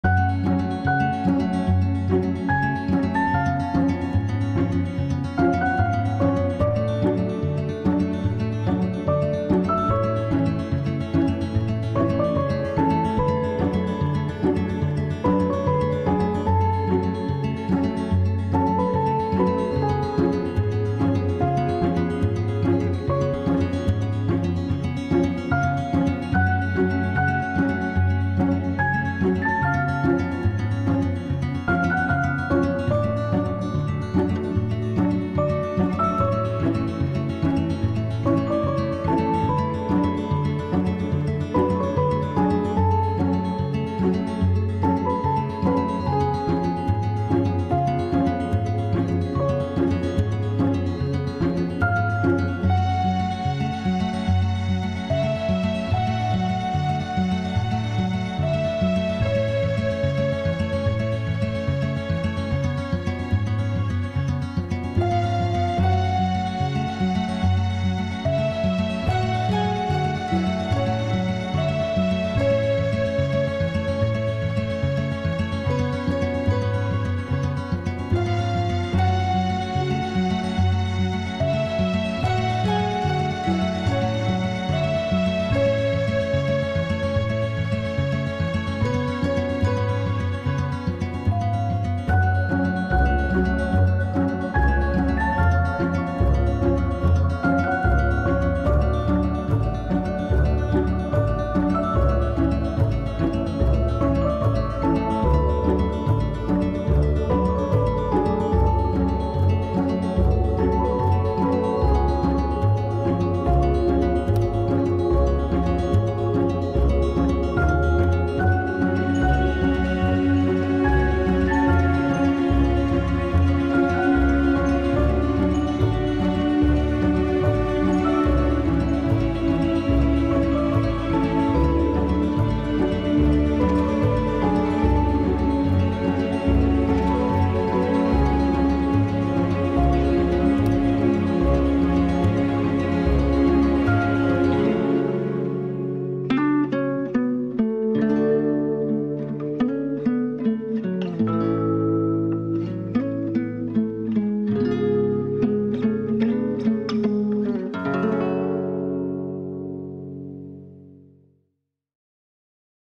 TIMELAPSE DRAWING | Timothy Spott Casual Doodle
Been kinda taking a small break from making art and timelapse art videos. Thought I'd do a little doodle for my OC Timothy Spott.